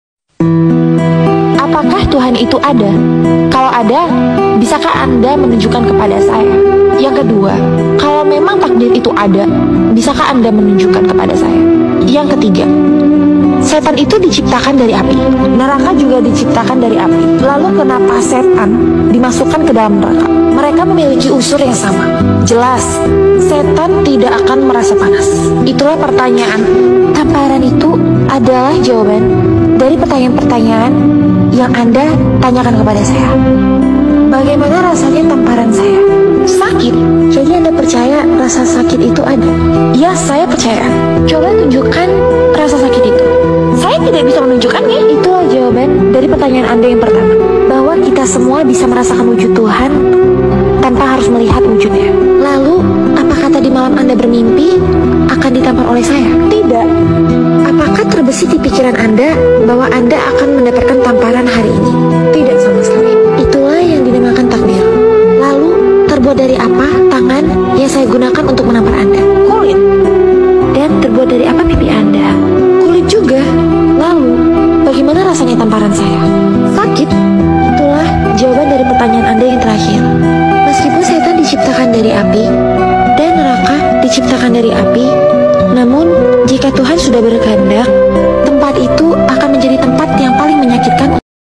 Mengurai tiga pertanyaan tentang tauhid my Video#Viral
Pertanyaan Tauhid perlu untuk kita renungkan" Tiga pertanyaan satu jawaban, hal ini penting untung menambah hasanah keilmuan kita dengan ke ean Allah. Swt sang pemcipta